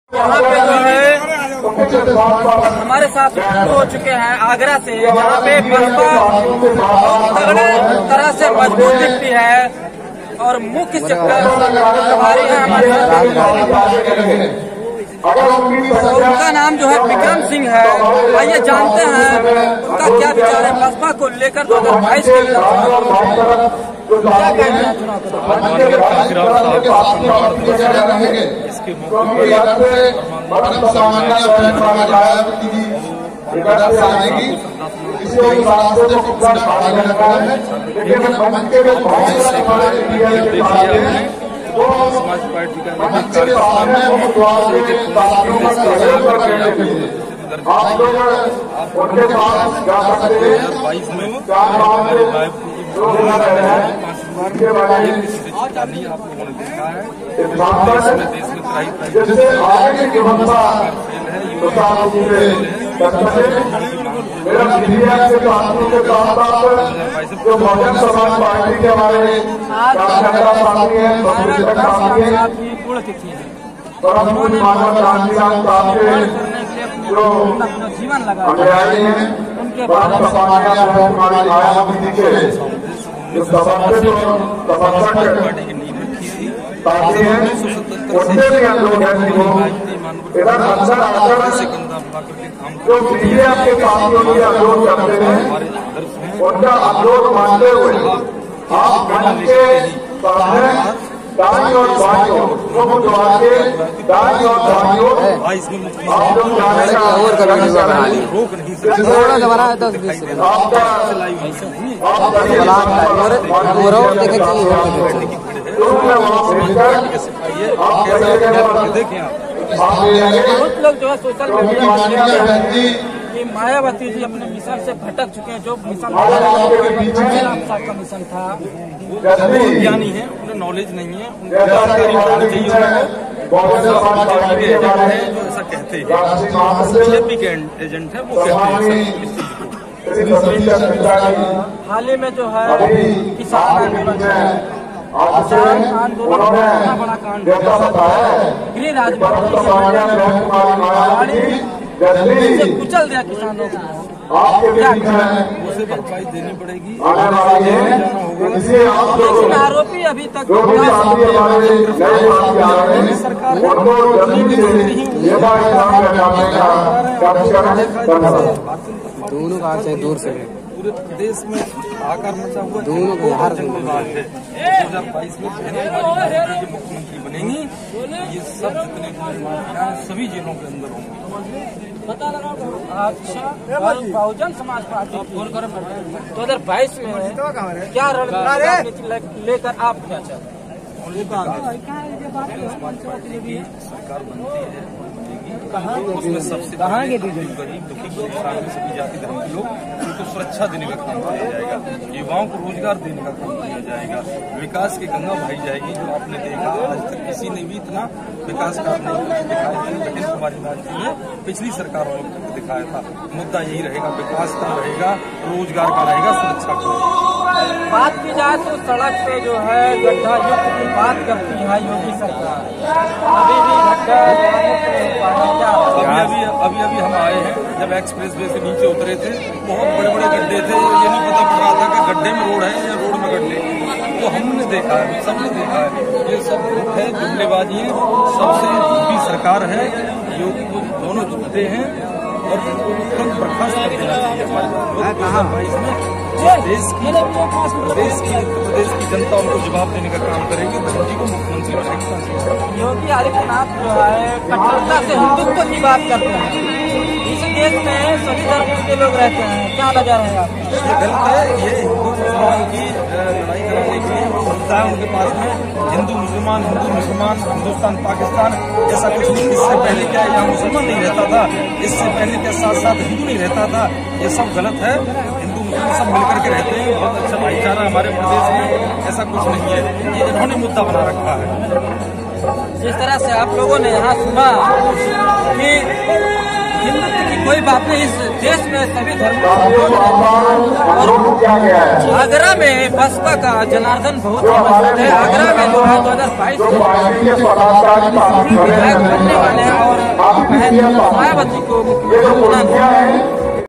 2022 में बसपा की सरकार बनी तो विकास की गंगा बहेगी- महेश कुमार
2022 में बसपा की सरकार बनी तो विकास की गंगा बहेगी- महेश कुमार